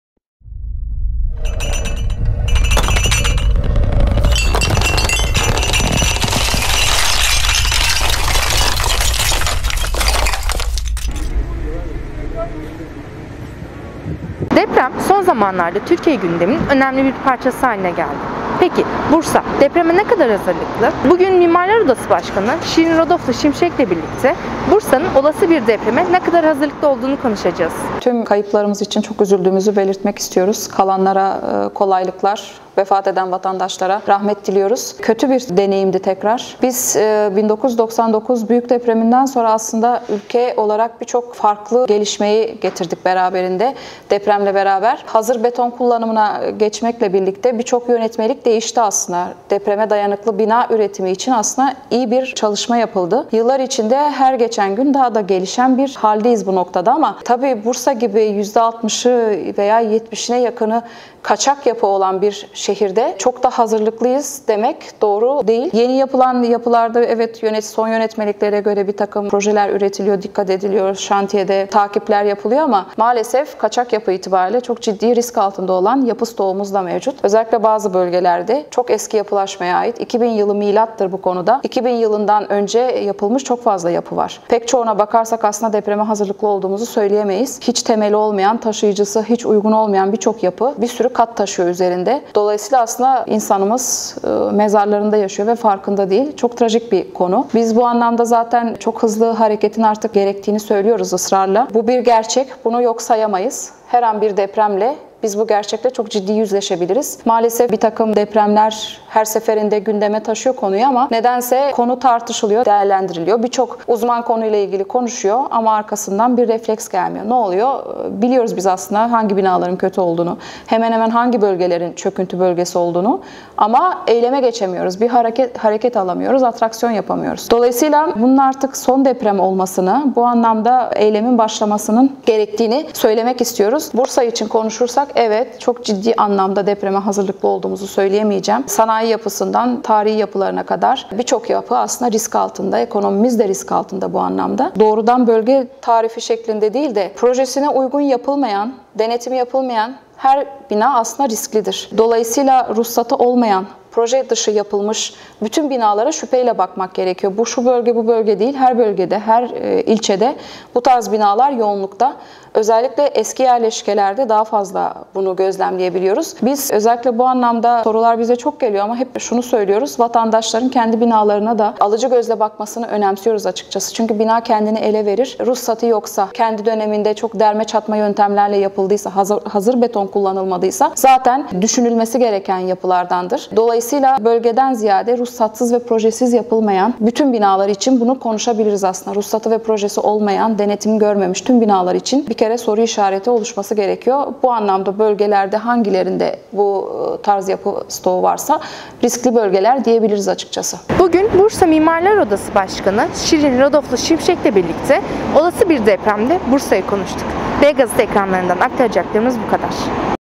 Bursa olası bir depreme hazır mı?
Türkiye'de son zamanlarda art arda yaşanan depremler gündemin büyük bir parçası haline gelirken, gözler Kuzey Anadolu Fay Hattı’nın üç kolu ile çevrelenmiş Bursa’ya çevrildi. Bursa Mimarlar Odası Başkanı Şirin Rodoplu Şimşek, BGazete mikrofonuna olası bir depremde Bursa'yı anlattı.